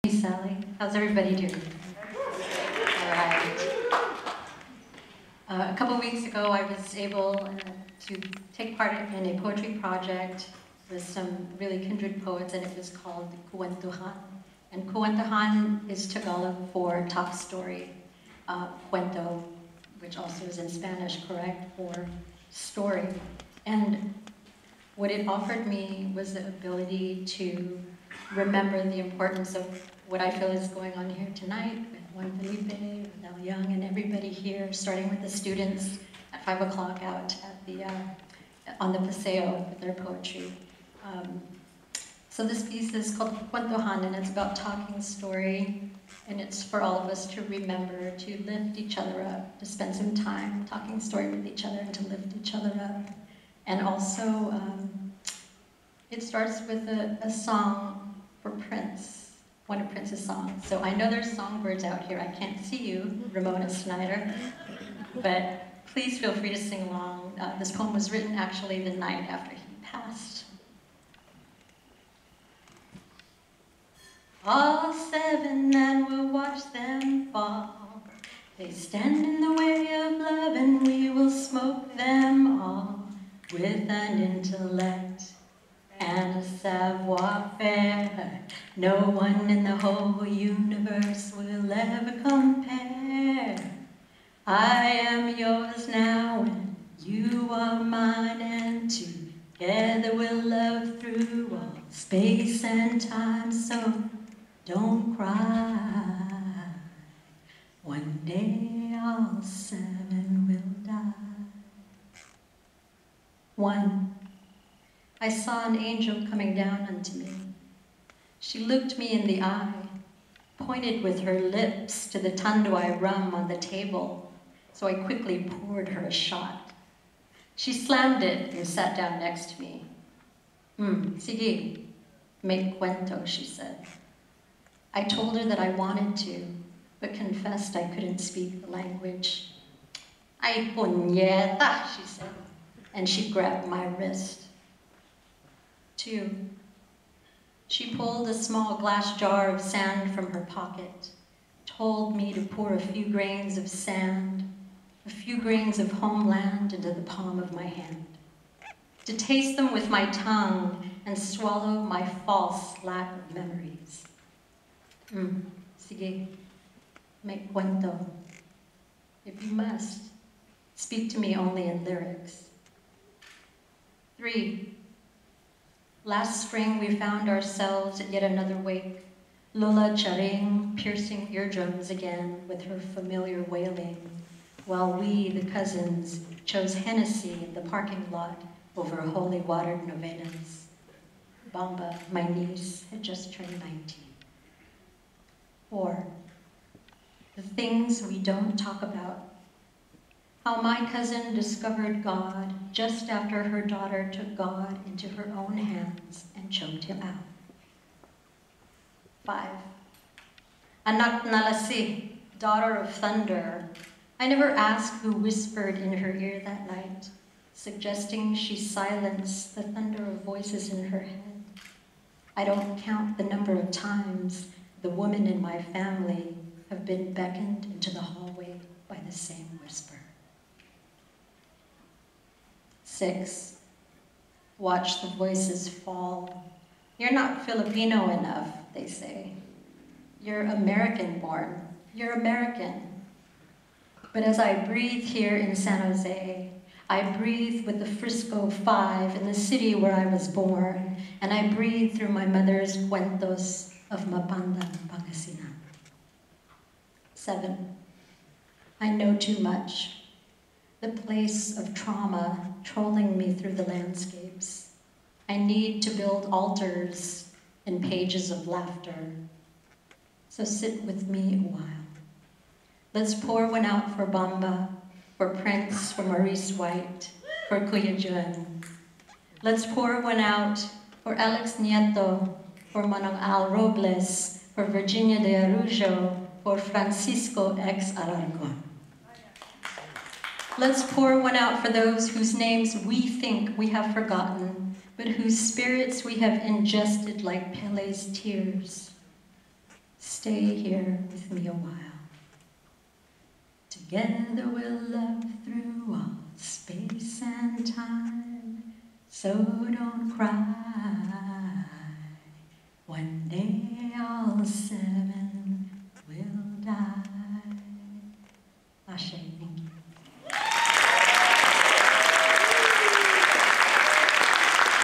Arlene Biala, Poet Laureate of Santa Clara County 2016-20017
At the SJSU Legacy of Poetry Day Celebration 2016, at Hammer Theatre with Juan Felipe Herrera